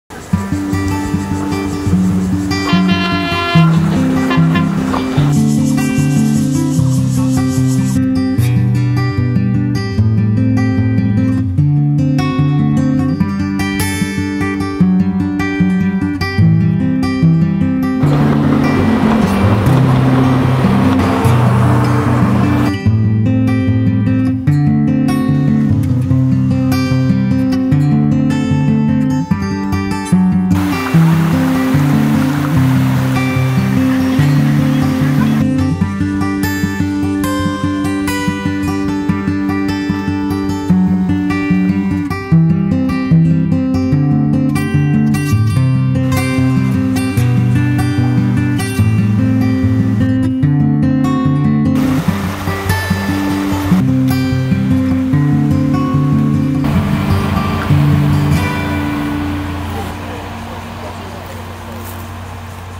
獨立山森林鐵路健走步道，樟腦寮車站，Chiayi travel
嘉義獨立山步道由樟腦寮車站開始，途中會經過森林鐵路，運氣好還能觀賞火車穿越森林的畫面～